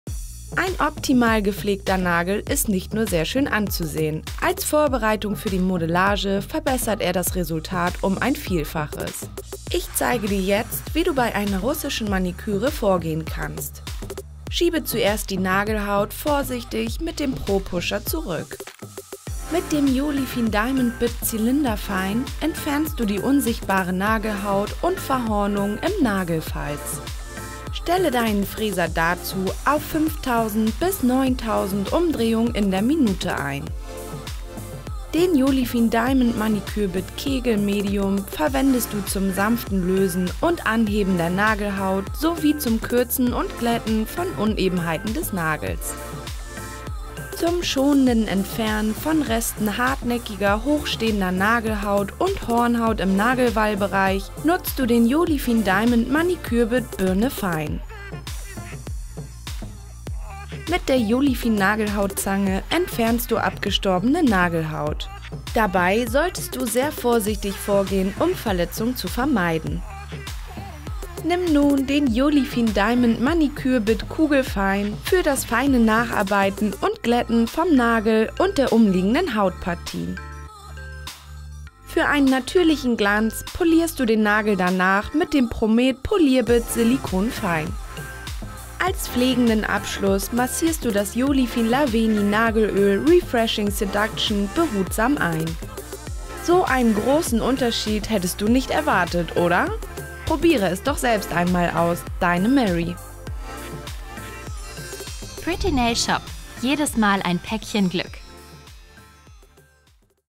TrendStyle Fundstück „Russische Maniküre“
Ob Gelmodellage oder Lackauftrag - das A und O dabei ist die professionelle Bearbeitung und Entfernung der Nagelhaut. Mit einer russischen Maniküre schaffst Du die perfekte Grundlage für makellose Fingerspitzen. Hier gelangst Du zu den Produkten